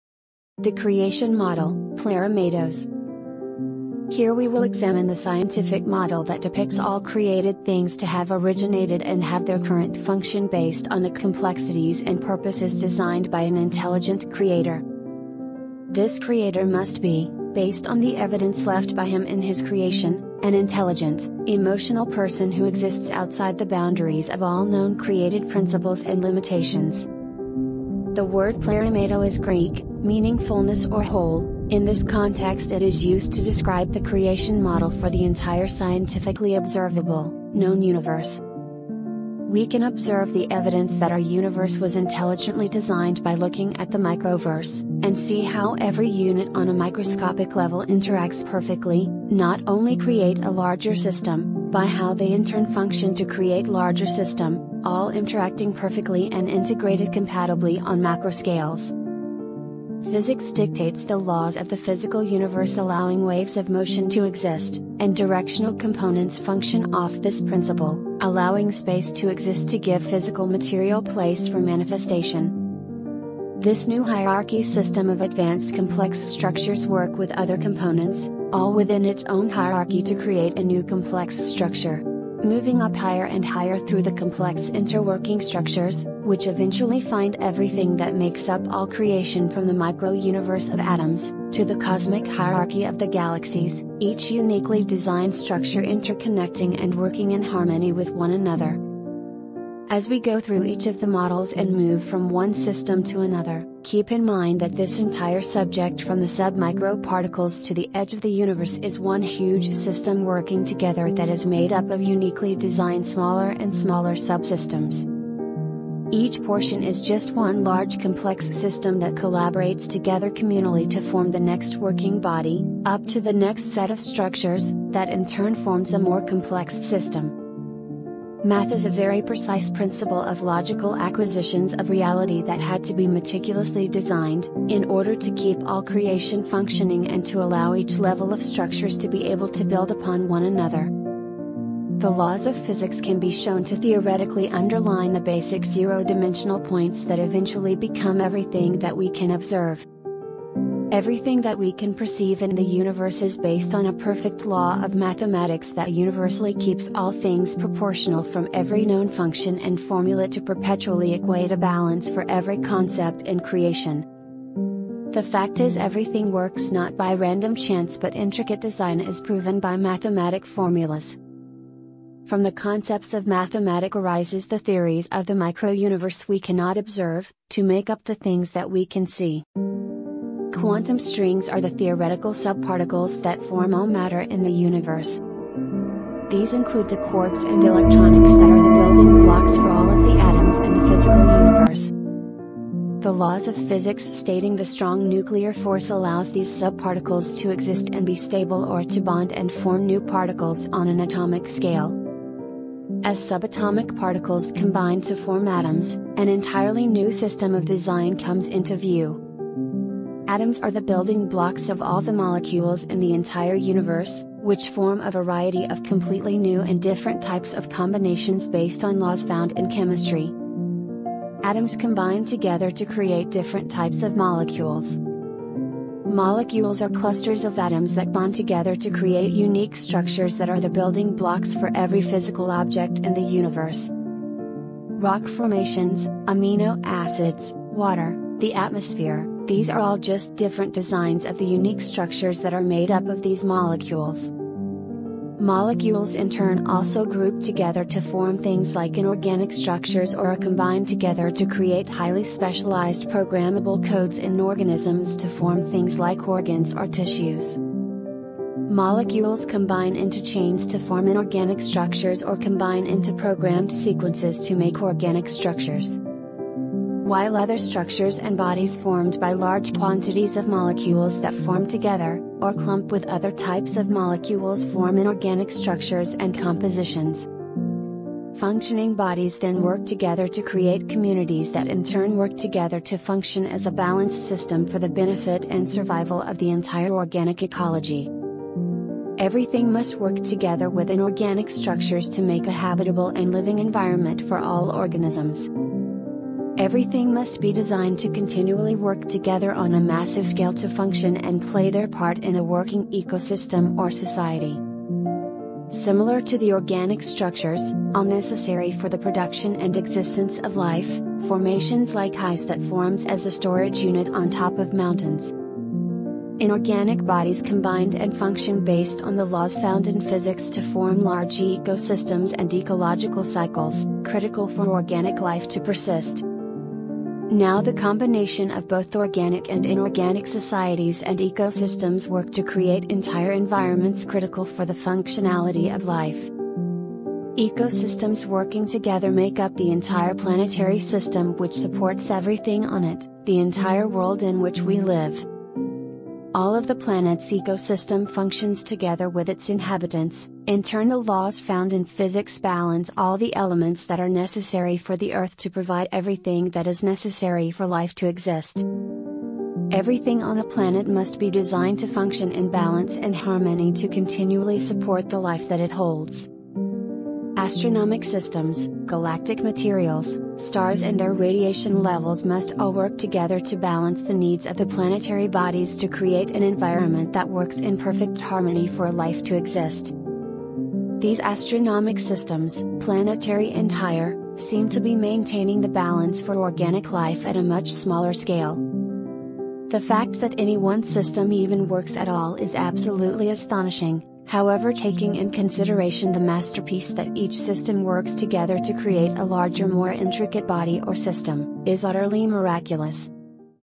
The Creation Model
Intelligent Design is the scientific model that depicts all created things to have originated and have their current function based on the complexities and purposes designed by an intelligent Creator.